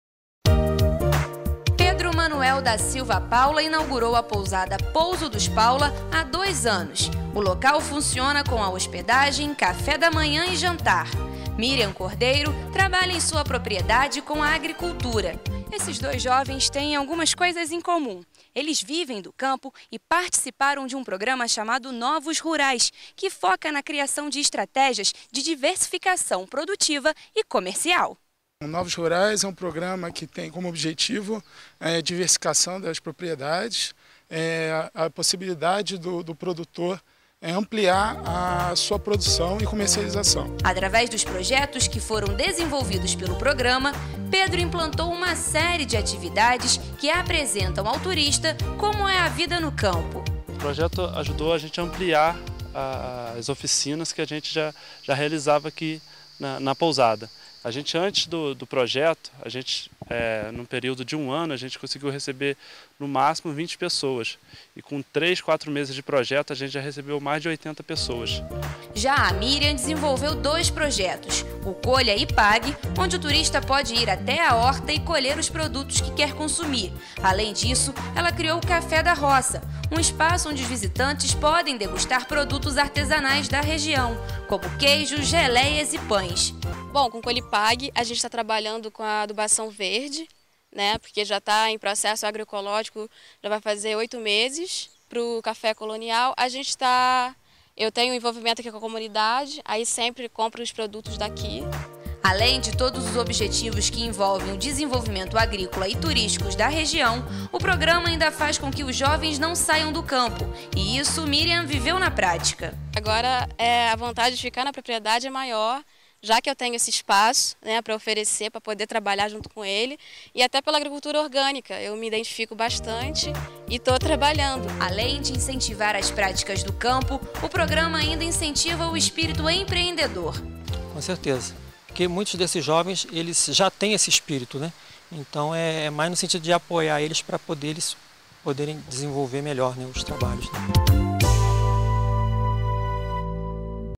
Nova Friburgo incentiva jovens em projetos de turismo e desenvolvimento rural
Em Nova Friburgo, um programa da Secretaria Estadual de Agricultura e Pecuária incentiva os jovens a não saírem do campo investindo em projetos de turismo e desenvolvimento rural. Na localidade de Três Picos, a ideia deu certo e já rende lucro.